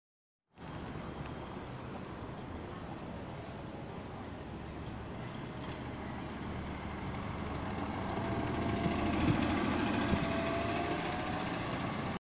SF Muni 2015 New Flyer XDE60 6538 on Route N
Engine: Cummins ISL9
Transmission: Allison H 50 EP
Division: Islais Creek
Route: N-Judah
Direction: Inbound to Caltrain